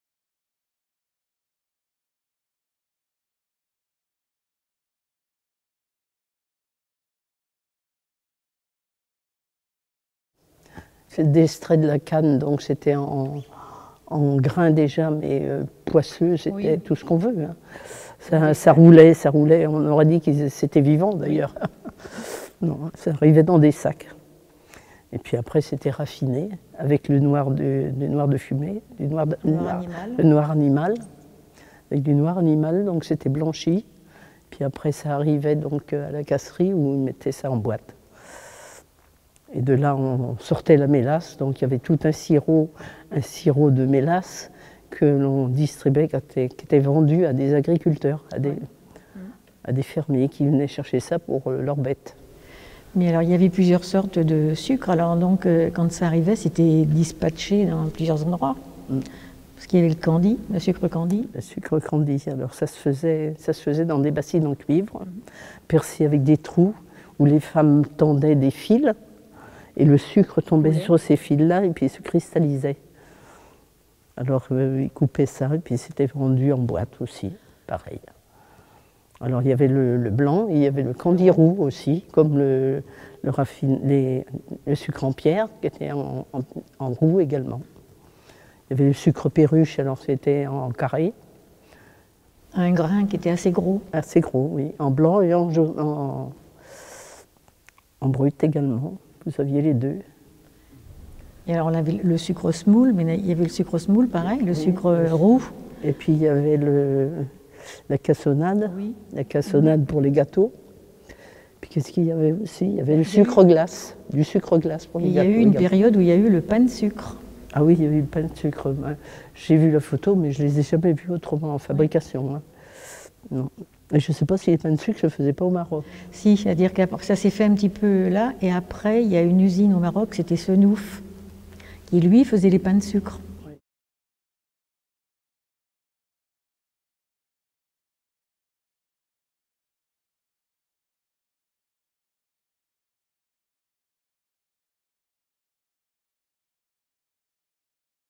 Témoignage : raffineries de sucre et techniques de fabrication à Chantenay
Mme Juliette Guérinau et Mme Jacqueline Ricordeau, anciennes employées de la Raffinerie de Chantenay, évoquent la fabrication des différents sucres, soit : le sucre Candy, la Perruche, blanc ou roux, le sucre semoule, la cassonade, le sucre glace et le pain de sucre.

Vidéo réalisée dans le cadre de l'Inventaire du patrimoine culturel du Bas-Chantenay en 2012 en partenariat avec le service patrimoine de la Région Pays de la Loire.